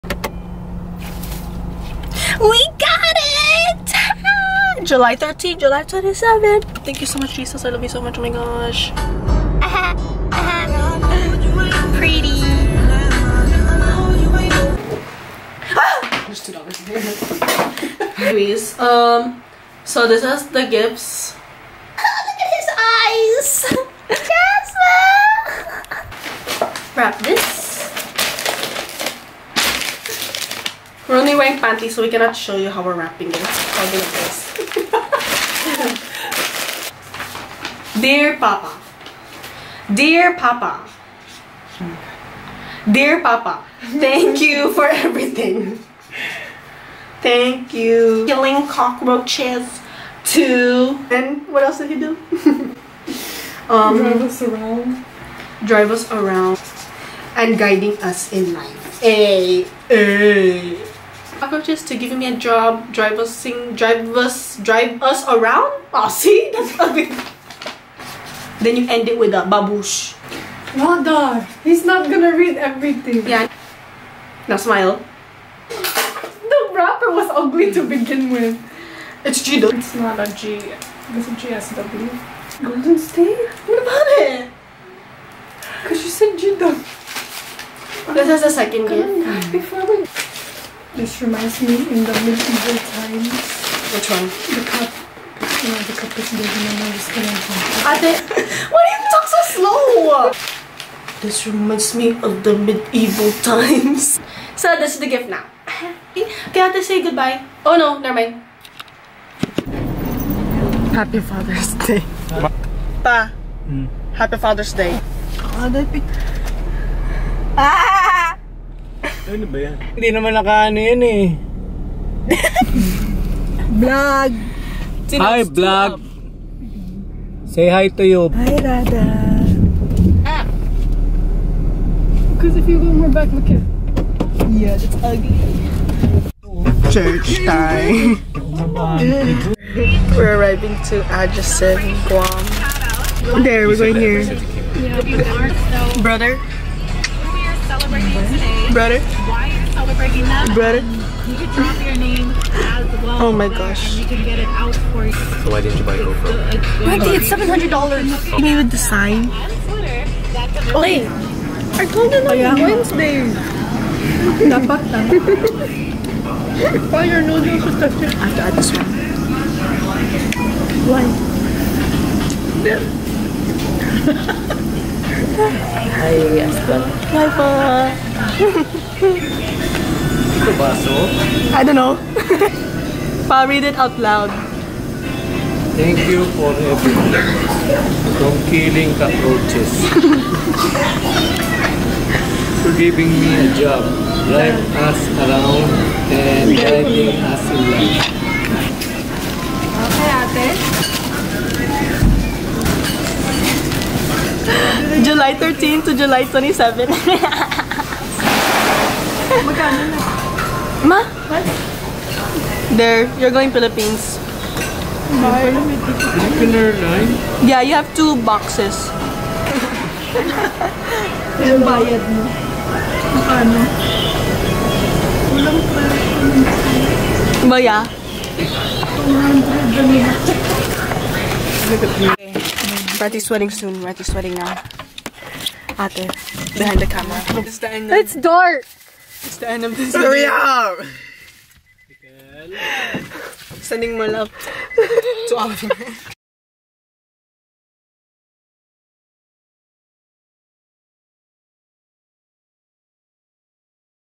Father's Day 2019
A very special father's day to me because I got to buy my dad's plane ticket to Philippines with money I had saved up!!

Always and forever thankful for Jesus